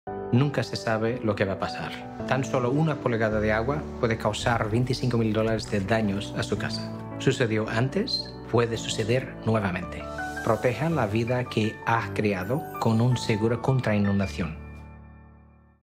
La Historia de un Sobreviviente: Paulo Pires (15 sec)
Como un agente de seguro contra inundación, Paulo Pires le vende el seguro contra inundación a sus clientes para cuidarlos. Después de la devastación del huracán Harvey en Texas, los clientes de Paulo pudieron recuperarse y reconstruir gracias al Programa Nacional de Seguro contra Inundación.